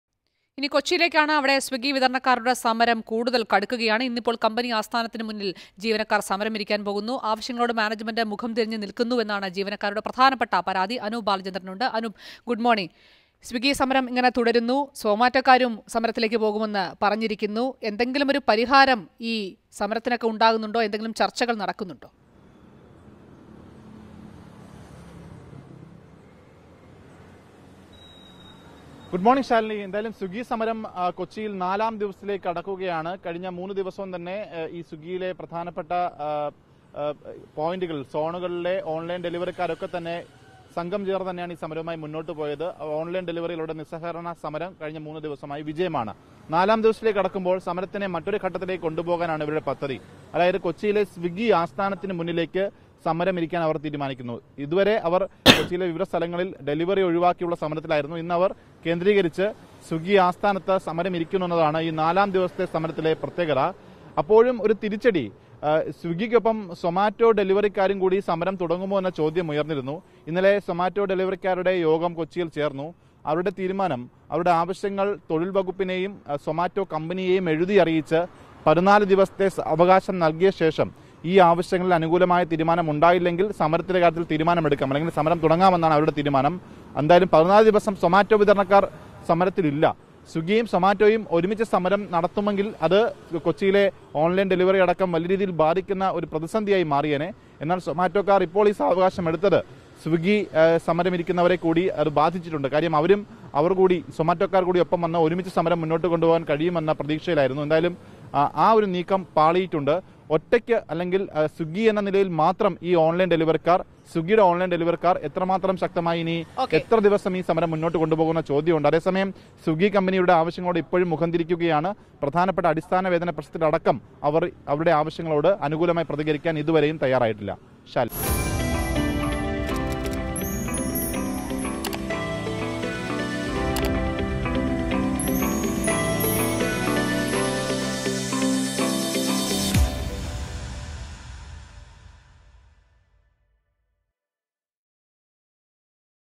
സ്വിഗ്ഗി വിതരണക്കാരുടെ സമരം തുടരുന്നു ; ഇന്ന് കമ്പനി ആസ്ഥാനത്തിനു മുന്നിൽ സമരമിരിക്കും | Swiggy
സ്വിഗ്ഗി വിതരണക്കാരുടെ സമരം തുടരുന്നു ; ഇന്ന് കമ്പനി ആസ്ഥാനത്തിനു മുന്നിൽ സമരമിരിക്കും